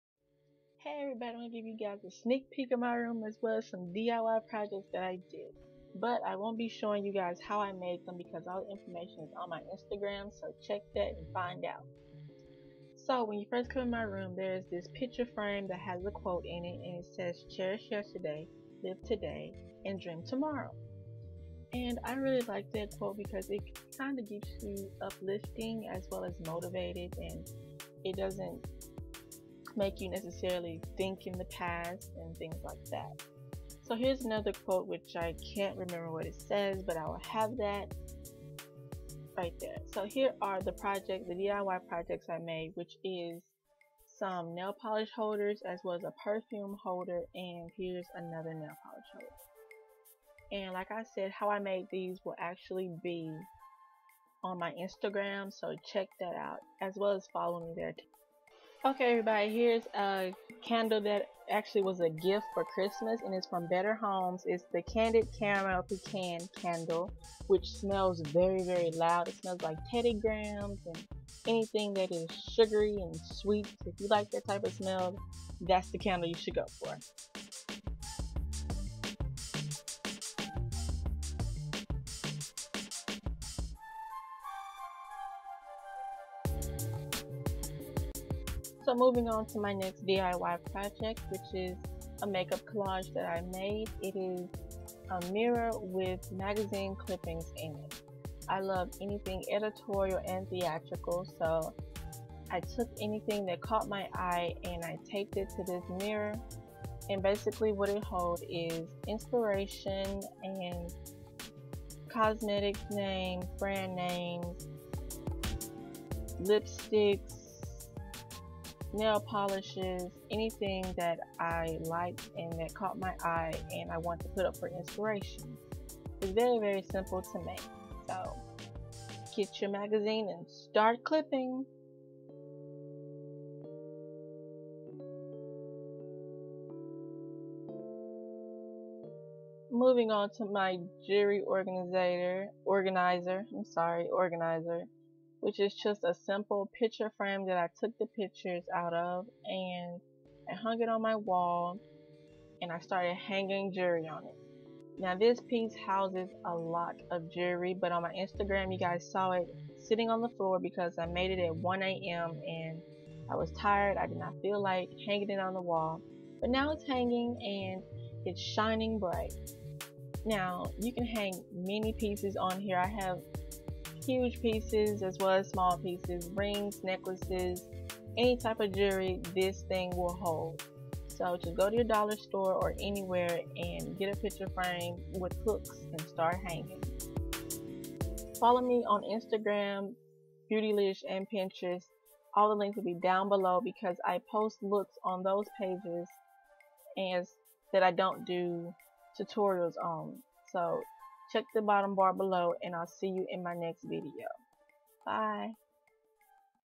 D I Y Projects + Sneak Peek of My Room
MARCH MADNESS.. This video DOES NOT show you how to make these pieces, info is on my Instagram so follow me there for more diy projects. Wanting to give you guys a little insight of my room. I wanted to give back all the inspiration that i received from everyone by doing a D.I.Y Project myself and showing you.